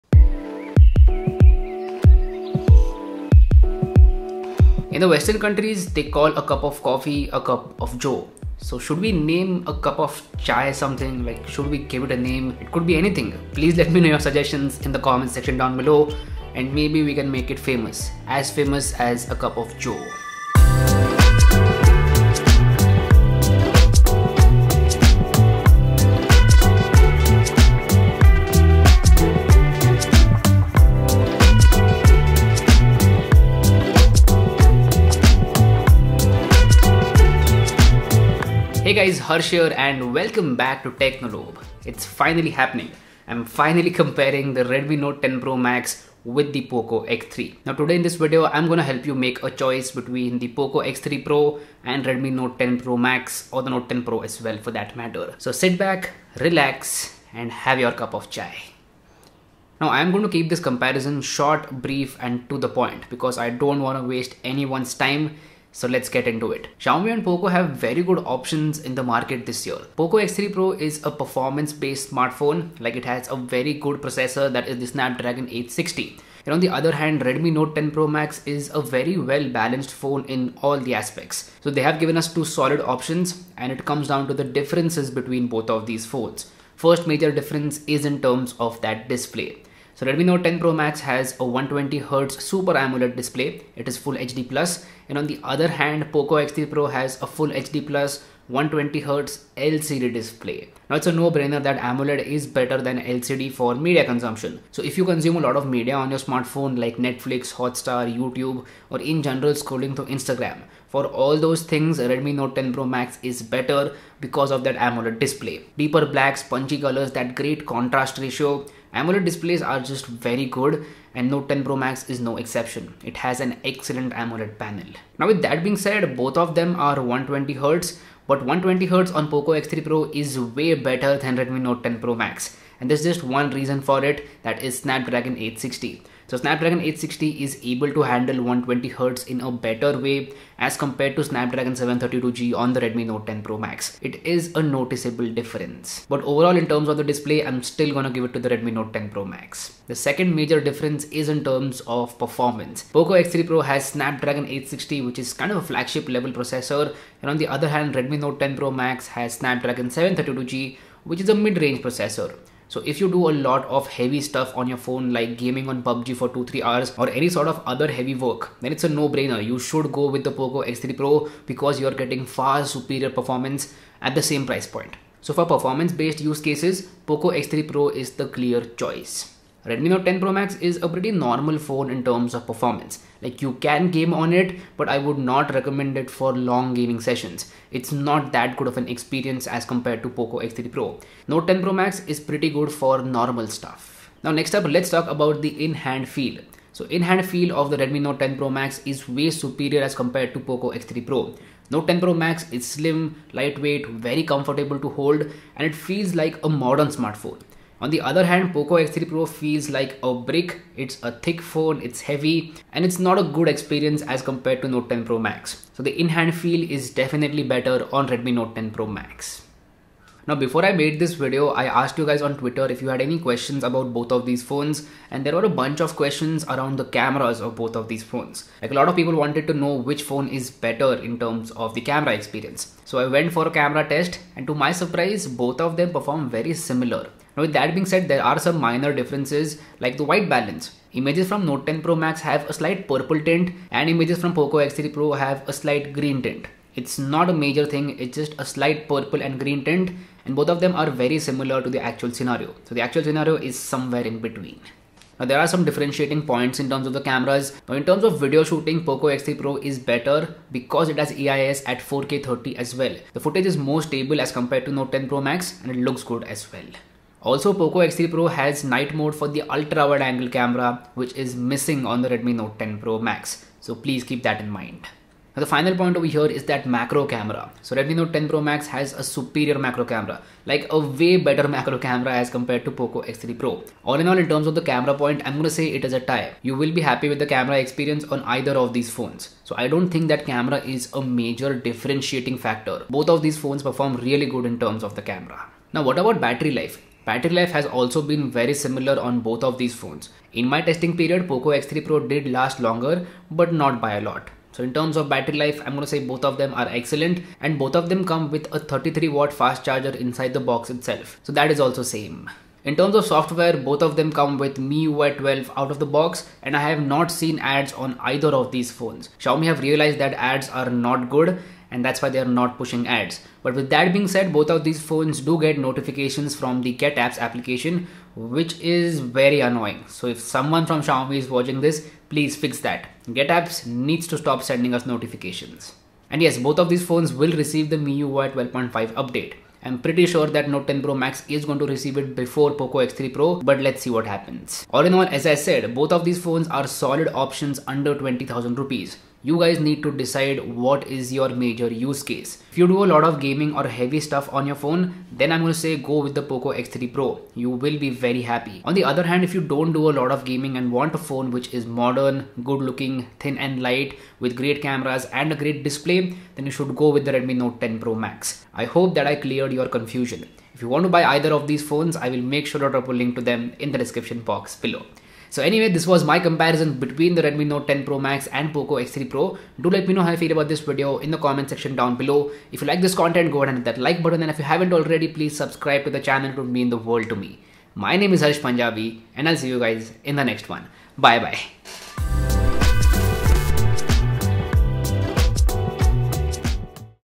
Poco X3 Pro vs Redmi Note 10 Pro Max Full Comparison | Which one to get?!
Poco X3 Pro and Redmi Note 10 Pro Max are both really good smartphones under Rs.20,000. Today in this video let u compare the Poco X3 Pro and Redmi Note 10 Pro Max and let's see which one is better for you.